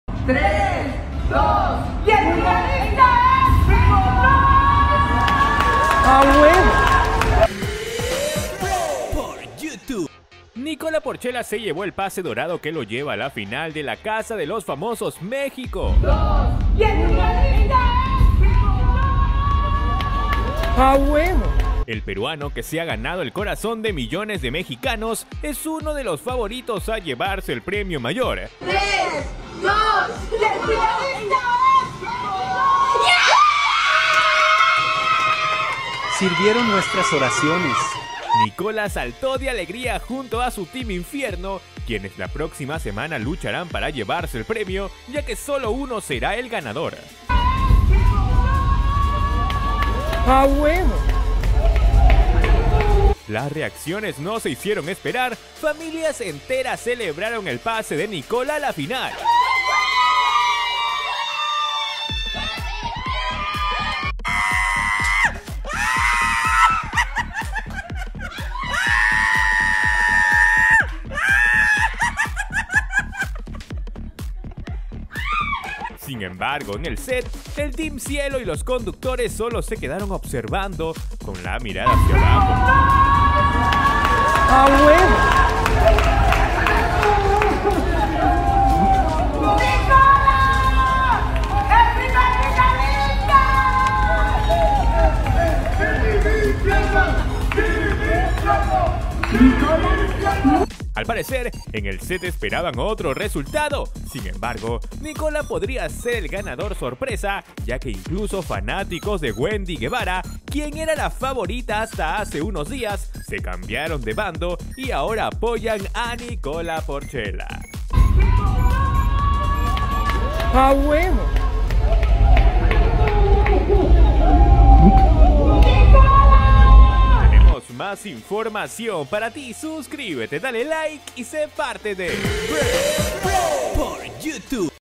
NICOLA PORCELLA: REACCIONES DE MÉXICO A SU PASE A LA FINAL EN LA CASA DE LOS FAMOSOS
Nicola Porcella pasó a la gran final de la Casa de los Famosos México y así reaccionaron las familias mexicanas.